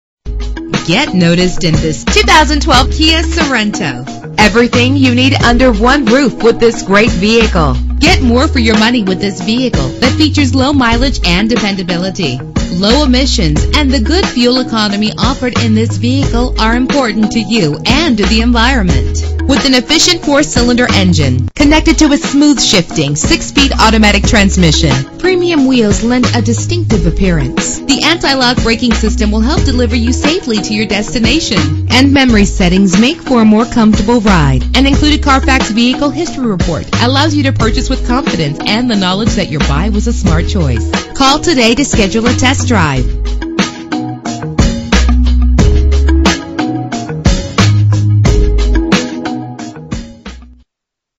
2012 Kia Sorento - Akron OH
Phone:
Year: 2012
Make: Kia
Model: Sorento
Trim: EX
Engine: 2.4 liter inline 4 cylinder 16 valve GDI DOHC
Transmission: 6-Speed Automatic
Color: Ebony Black
Mileage: 2559
Address:
1490 VERNON ODOM BLVD